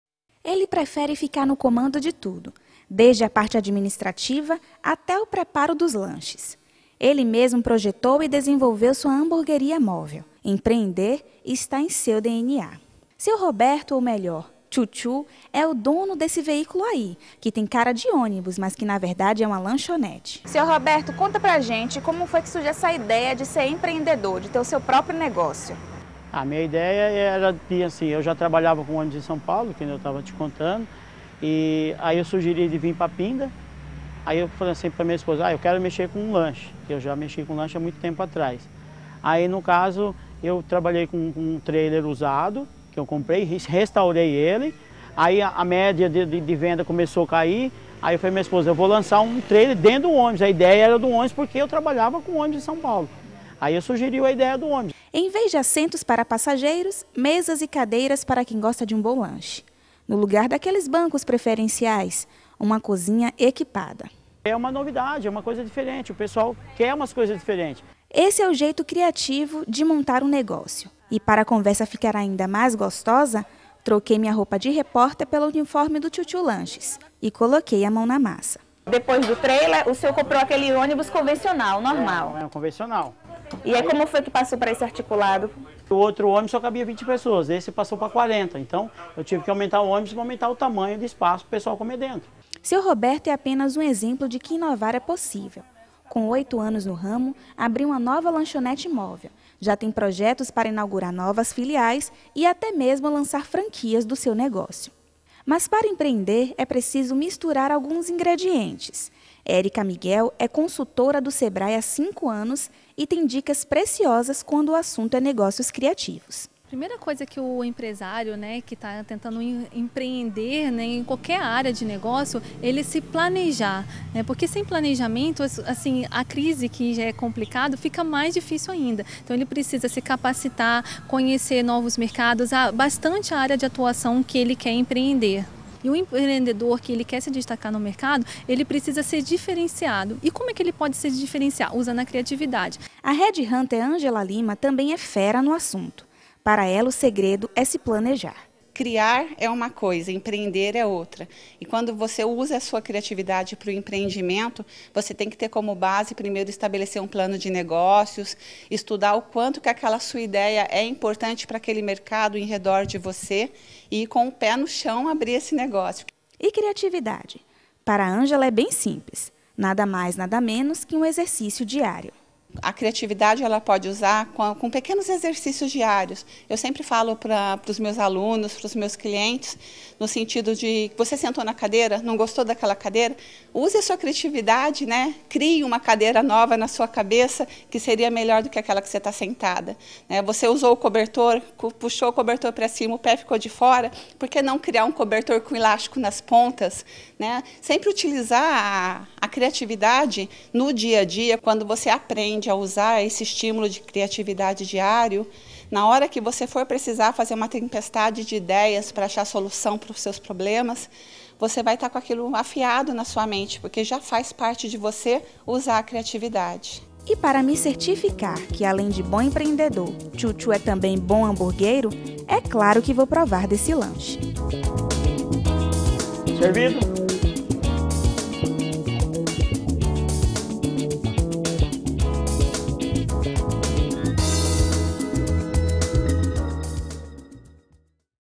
Criatividade para empreender
Que tal um toque de criatividade na hora de empreender? Veja como é possível vencer os desafios de ter um negócio usando a criatividade.